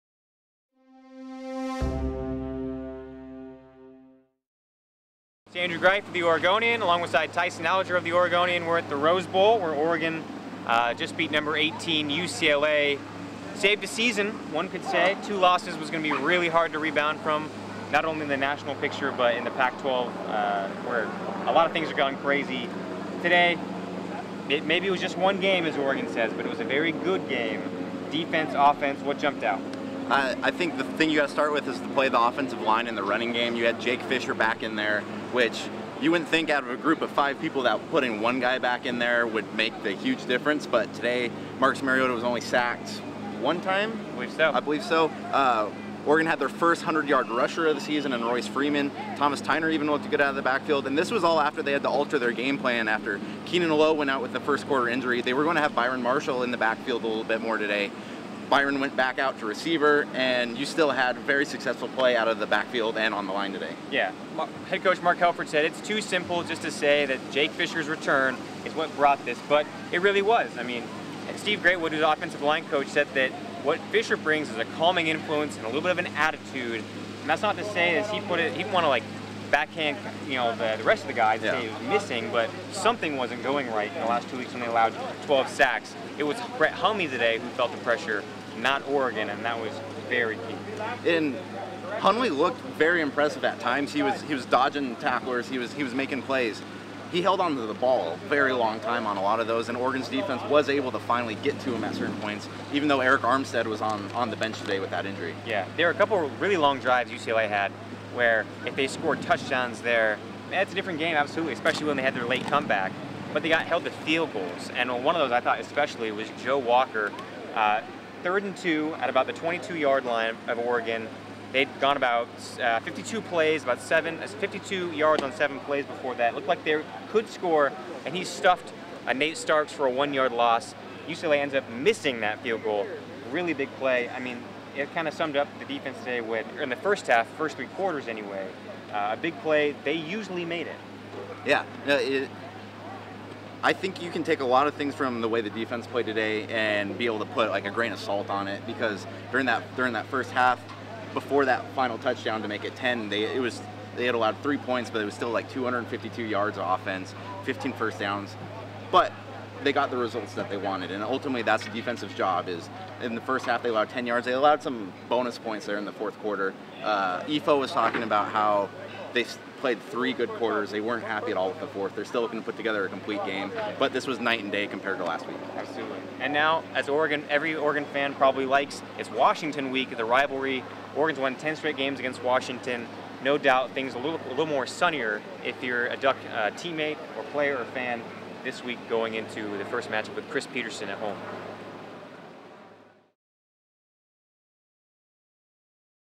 Oregon 42, UCLA 30: Tyson Alger and Andrew Greif talk about the win and what’s next for the Ducks
Does this win salvage Oregon’s season? Alger and Greif talk about the impact of the Ducks’ victory in Pasadena.